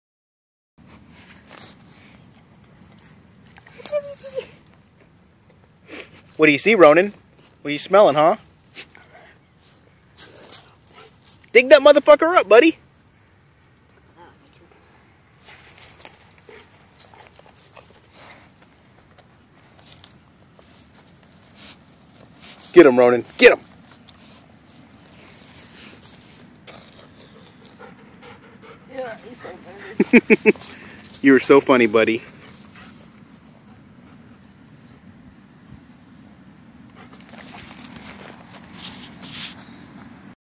German Shepherd Ronin digging again
Ronin trying to find the ever elusive squirrel